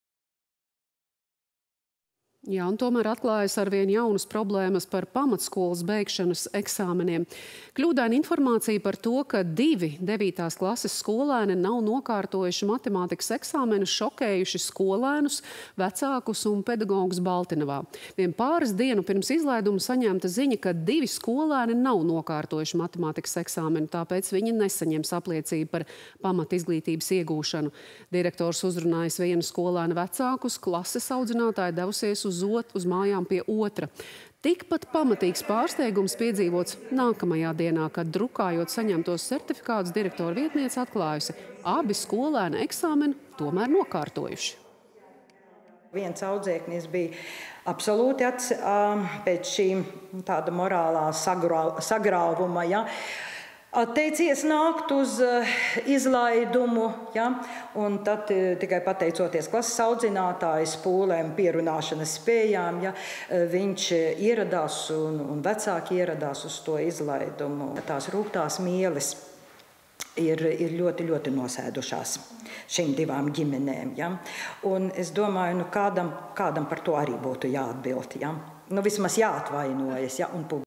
Skolēni sagrauti pēc maldinošiem eksāmenu rezultātiem
Un atklājas arvien jaunas problēmas ar pamatskolas beigšanas eksāmeniem... Kļūdaina informācija par to, ka divi devītās klases skolēni nav nokārtojuši matemātikas eksāmenu, šokējusi skolēnus, vecākus un pedagogus Baltinavā. Vien pāris dienu pirms izlaiduma saņemta ziņa, ka divi skolēni nav nokārtojuši matemātikas eksāmenu, tāpēc viņi nesaņems apliecību par pamatizglītības iegūšanu.